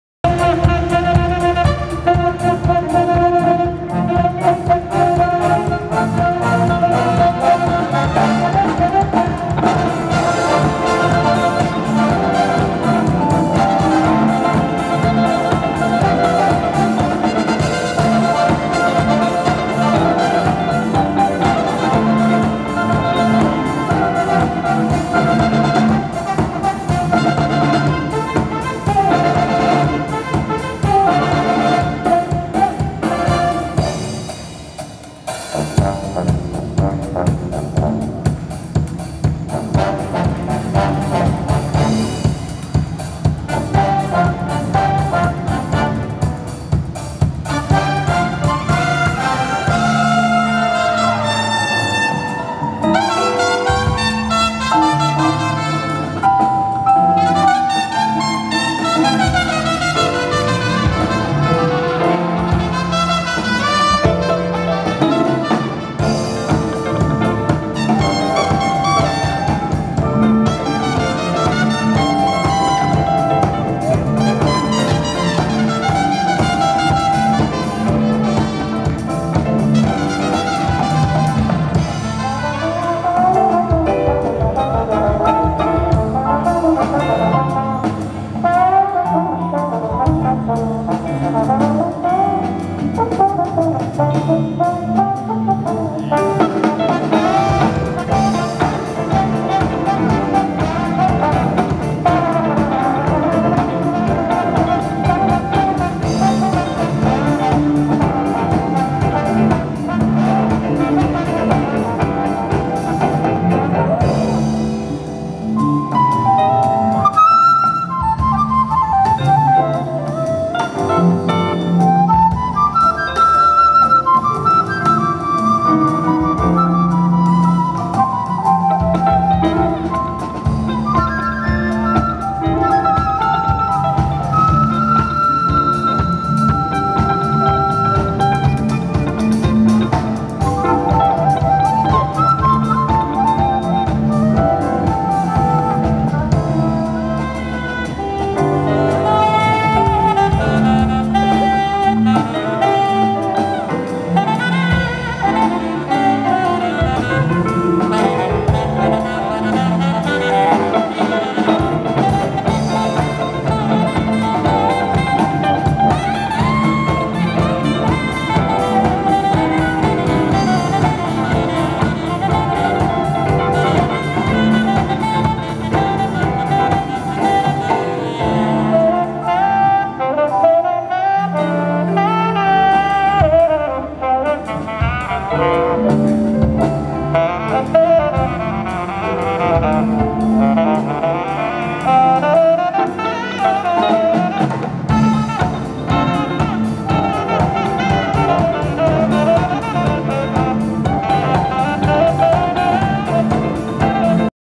Banda do 10º Batalhão de Juiz de Fora
Encontro de Bandas Militares 2016 no Cine-Teatro Central - Banda do 10º Batalhão de Juiz de fora.